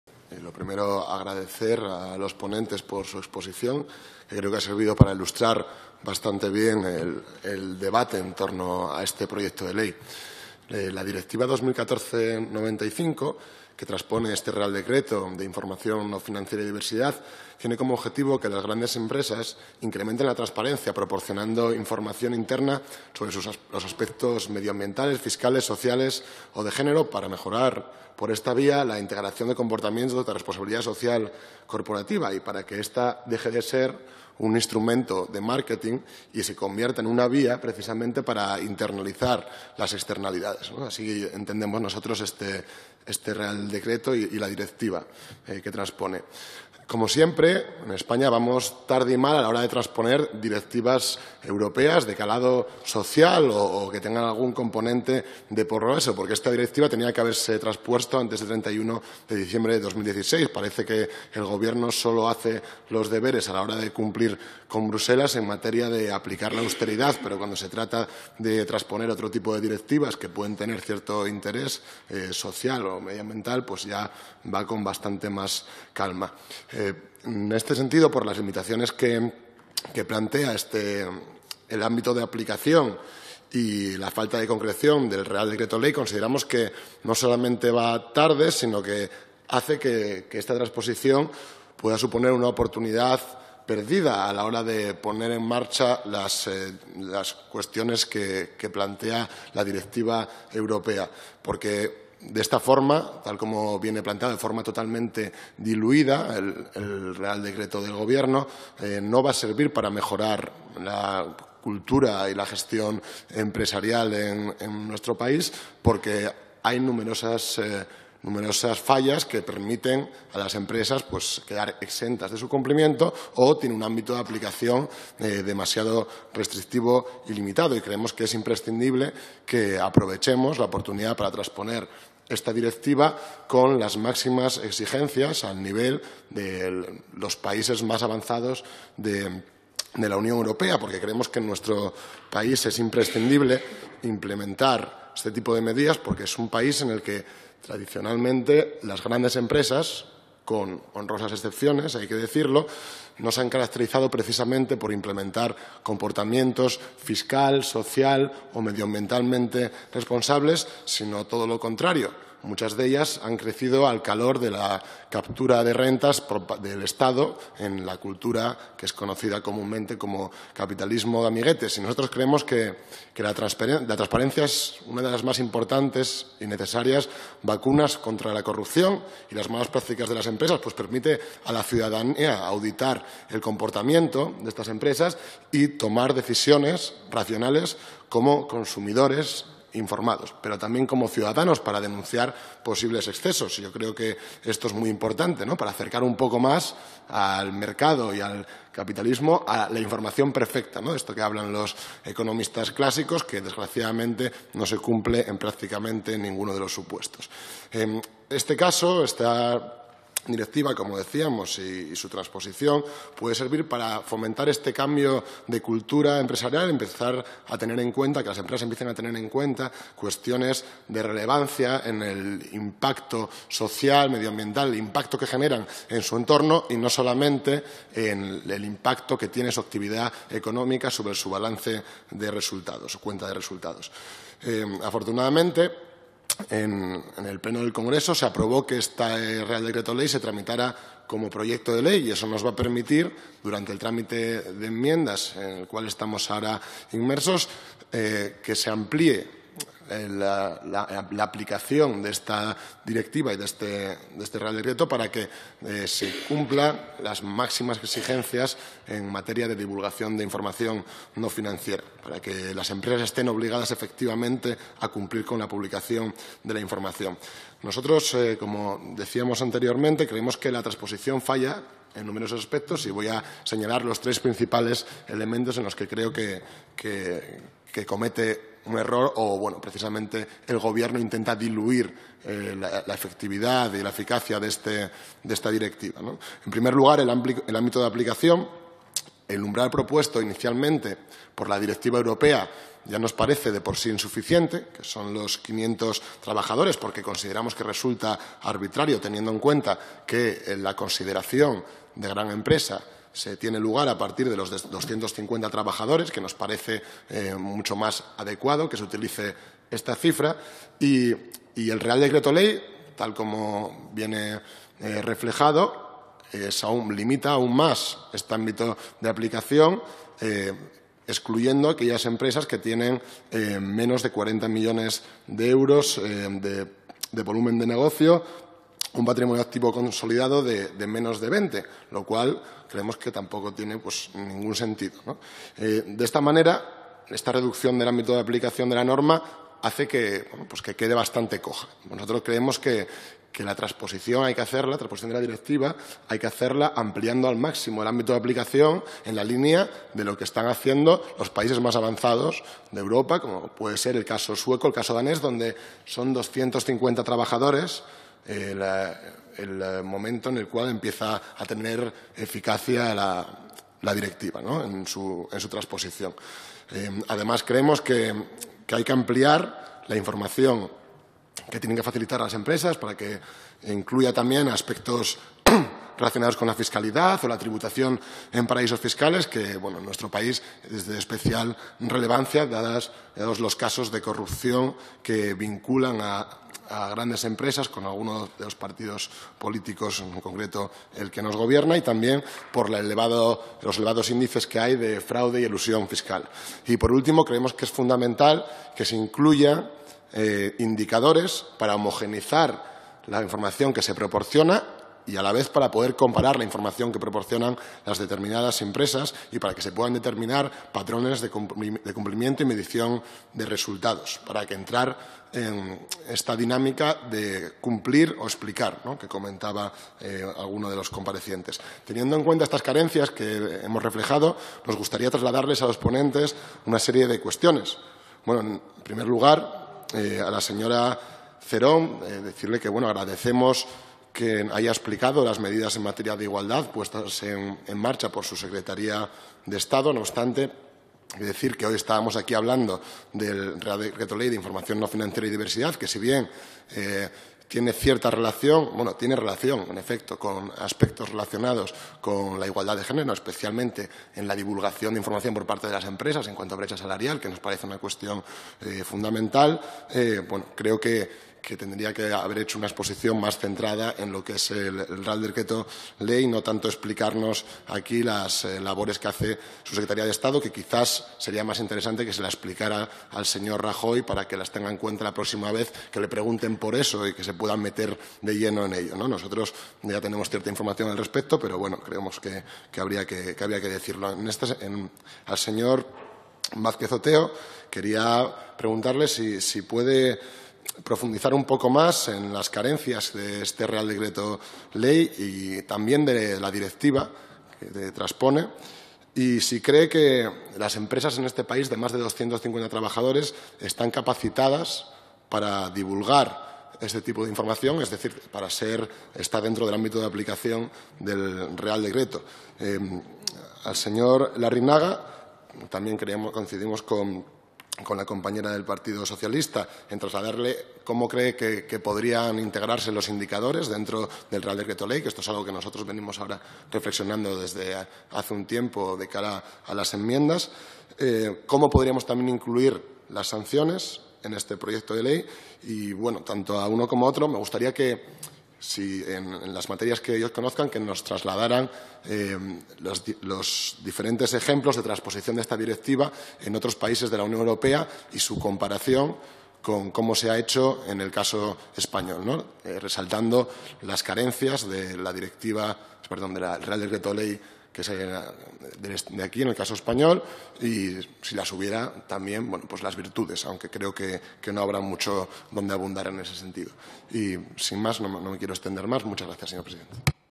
Segundo González en la Comisión de Economía, Industria y Competitividad el 26 de Febrero
Durante la celebración de diferentes comparecencias para informar en relación con el proyecto de ley por la que se modifica el código de comercio, el texto refundido de la ley de sociedades de capital aprobado por el real decreto legislativo 1/2010, de 2 de julio, y la ley 22/2015, de 20 de julio, de auditoría de cuentas, en materia de información no financiera y diversidad (procedente del real decreto-ley 18/2017, de 24 de noviembre).